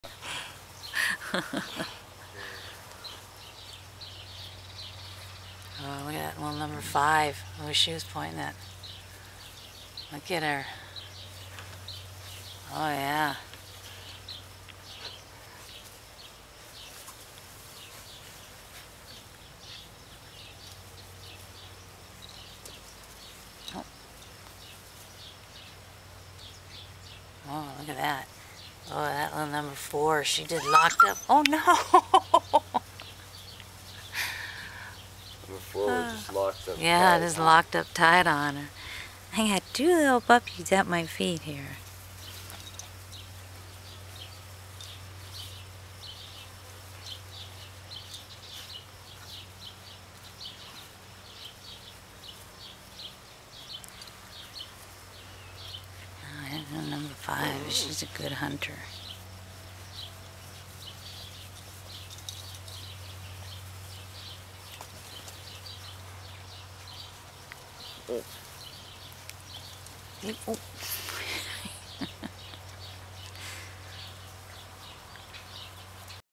Wirehaired Pointing Griffon puppies pointing - Jackie n Todd's Puppies on a bird wing
Wirehaired Pointing Griffon puppies pointing a bird wing.  These puppies are sold, but Jackie is expecting on St. Patricks Day, 2012.  Check our website for updates